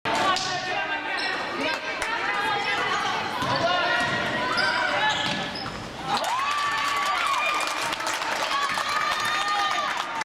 U13 T-Birds Basketball - Yelm Tourney - Championship Game (Olympia) - Jenna Ties the Score
Again, down two with about 45 seconds left...Antonia get the ball to Jenna at the top of the 3 point arc.  Jenna blows by Ellie and through two other Olympia Bears and gets off a running bank shot to tie the game.  Clutch drive and shot!